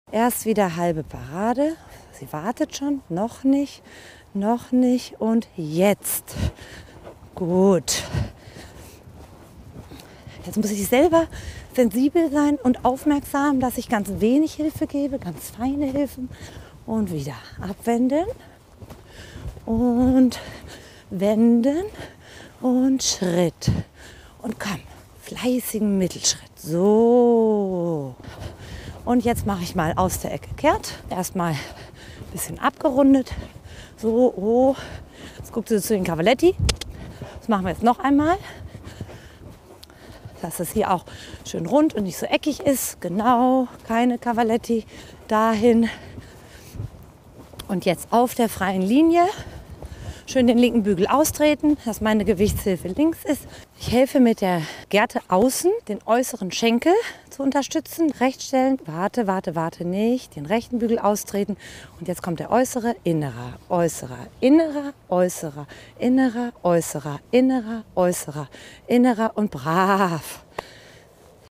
Das Erarbeiten von Schritt-Galopp-Schritt Übergängen & Kurzkehrt | Soma Bay | Ingrid Klimke
Ein Schwerpunkt dieses Trainings sind die Schritt-Galopp-Schritt Übergänge. Die Stute Soma Bay lernt Last aufzunehmen, eine Voraussetzung auch für das Erlernen des Außengalopps. Interessant ist zu beobachten, wie Ingrid Klimke ihre Hilfen immer mehr verfeinert. Dies gilt auch für die Kurzkehrt-Wendung.